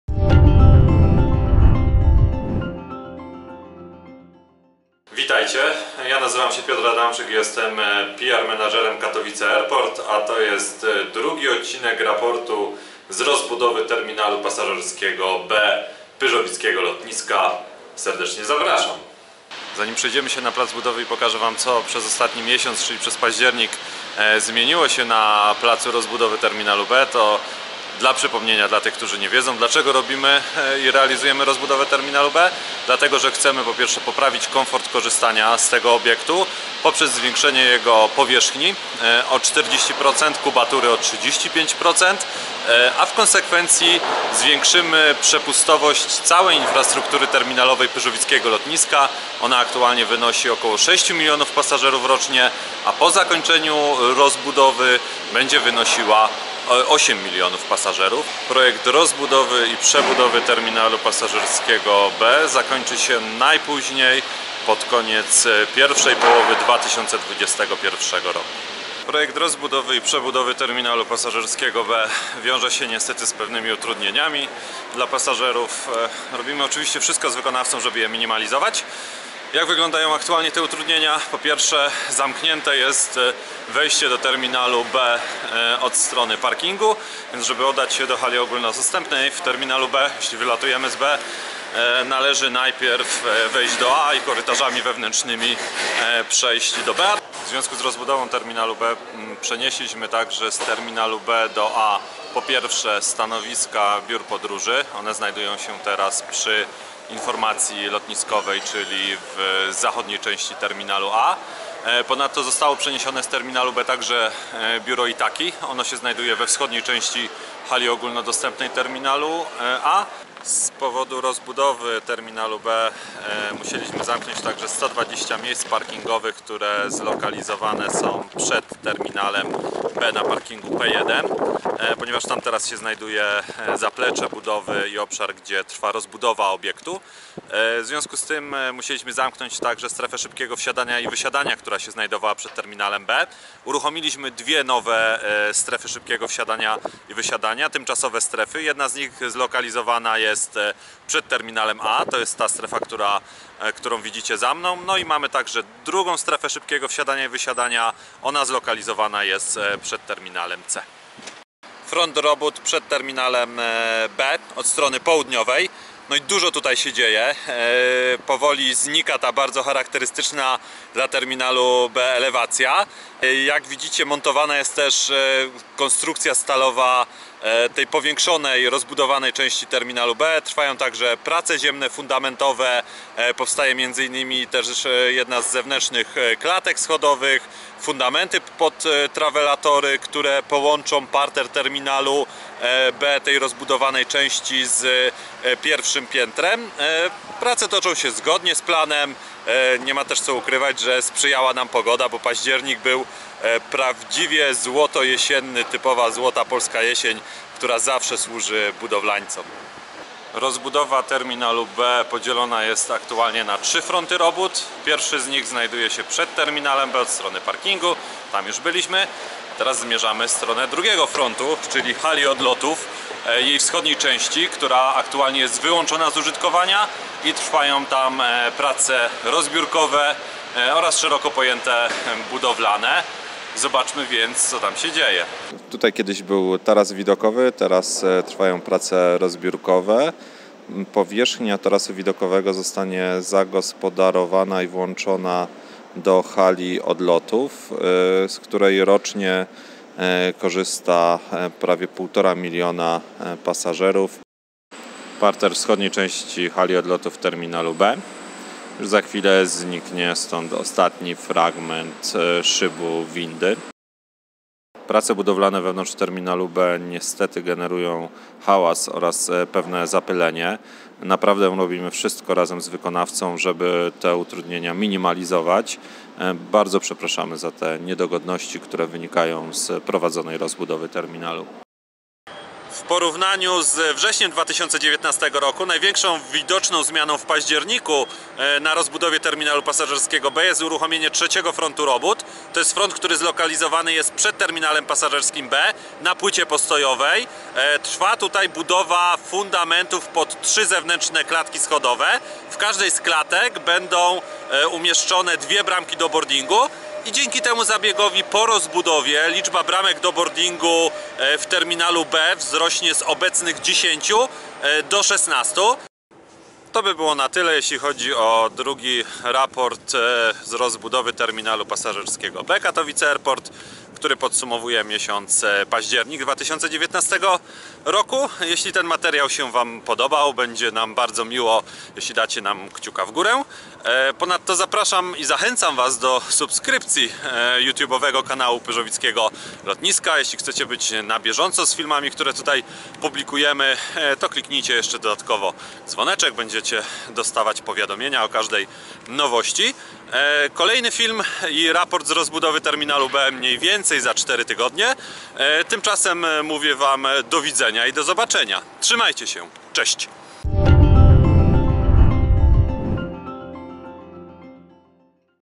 Październik 2019 | Raport z rozbudowy terminalu B Katowice Airport | VlogKTW 5
W Katowice Airport trwa duża i ważna inwestycja, czyli rozbudowa terminalu pasażerskiego B.

W drugim raporcie pokazujemy w jakich obszarach terminalu toczą się prace budowlane. Przedstawiamy także utrudnienia z którymi niestety muszą liczyć się podróżni odlatujący z pyrzowickiego lotniska.

Montaż: Piotr Adamczyk, PR Manager KTW
Ujęcia z drona: From the sky – Robert Neumann

Uwaga! Ujęcia z drona zostały wykonane za zgodą i pod nadzorem zarządzającego Katowice Airport oraz Polskiej Agencji Żeglugi Powietrznej. Informacje o tym jak latać bezpiecznie dronem